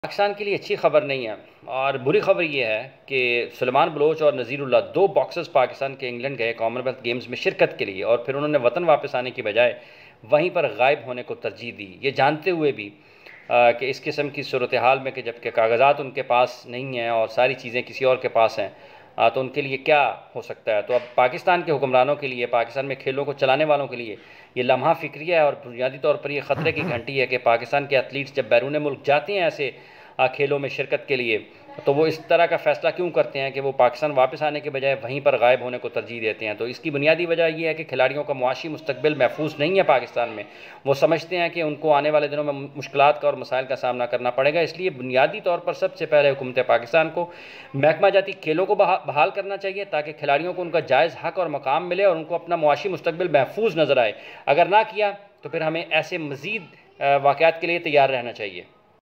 Bad news, two Pakistani boxers are missing in England
Bad news, two Pakistani boxers are missing in England, the authorities open their eyes, pay attention to the sport and players